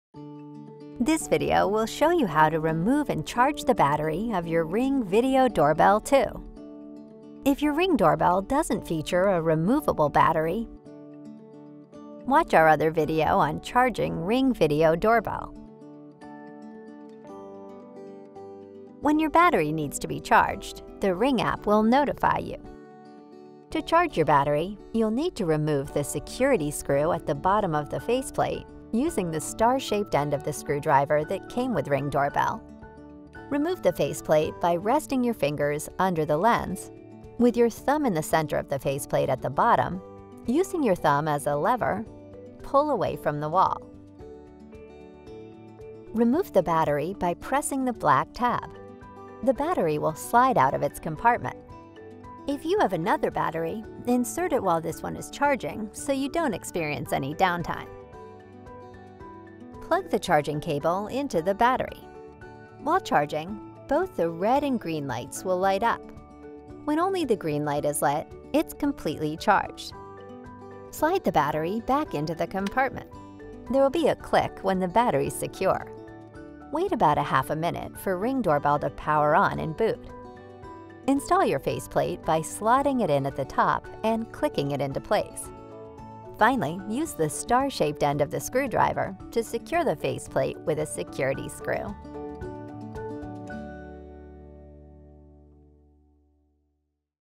How to Charge Your Ring Video Doorbell 2 Battery (Simple)
Learn how to charge the battery of Ring Video Doorbell 2.